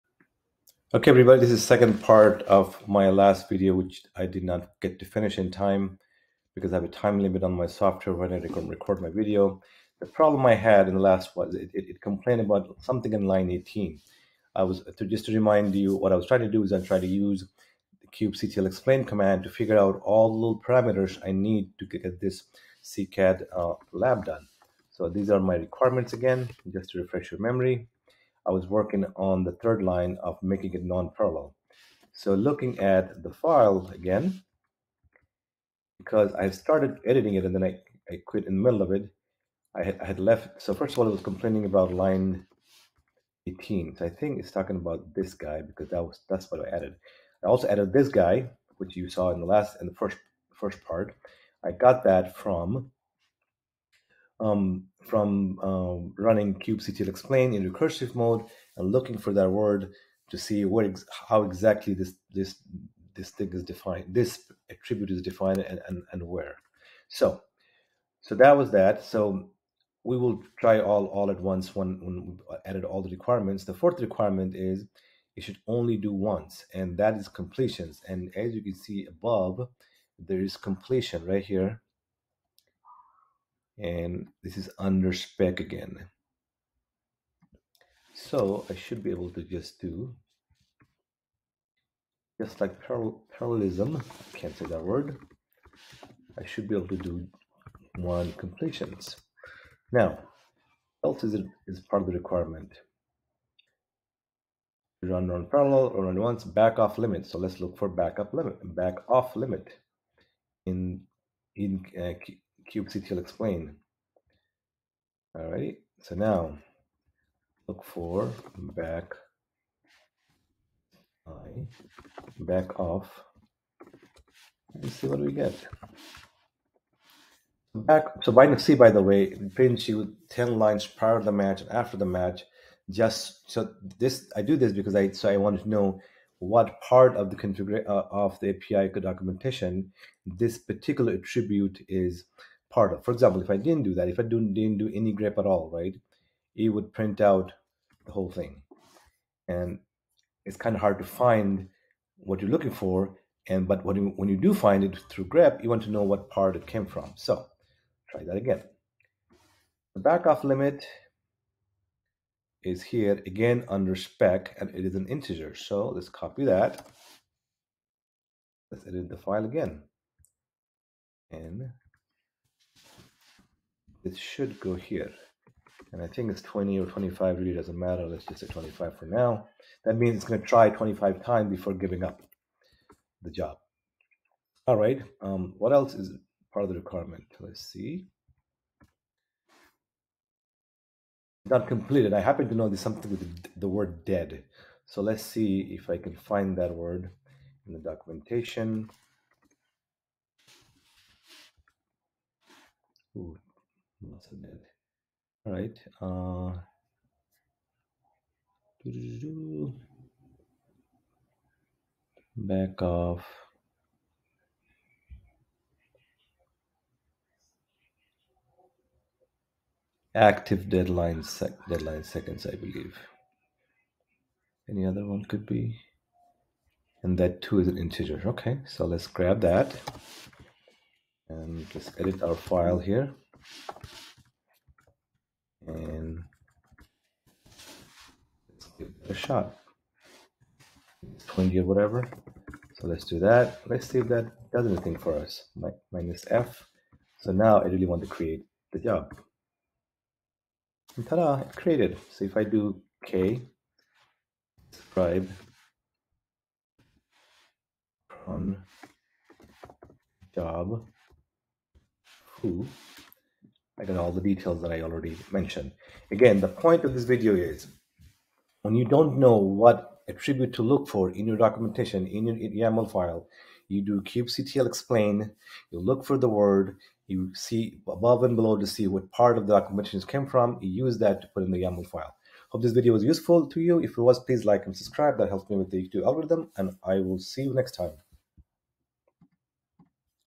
CKAD Lab 25 Magic of "kubectl explain" ! How to create YAML for a CKAD lab (cronjob) : Part 2
Next to kubectl -h, kubectl explain is your MOST powerful tool during your CKAD exam. In this demo (Part 2) , I show you how you can build an YAML file for a cronjob

Command:
kubectl explain cronjob --recursive | grep -i completion -C 10